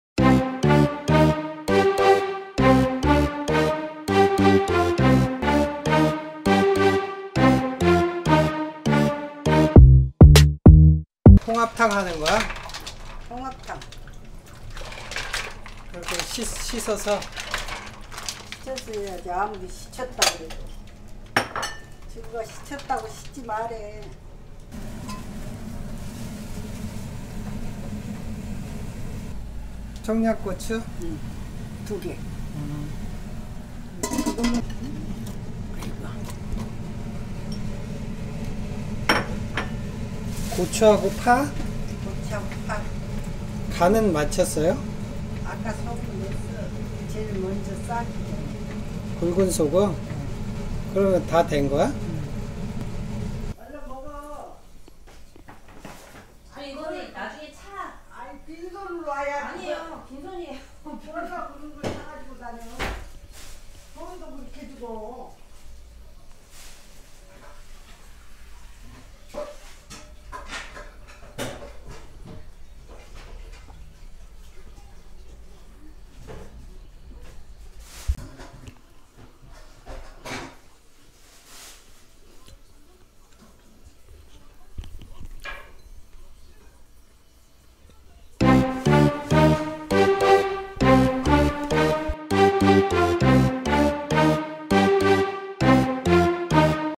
얼큰한 술국 홍합탕 끓이기
깨끗이 씻어놓은 홍합을 사다가,술국을 끓여봐요.
백합도 몇개넣으니 국물이 정말 시원하네요.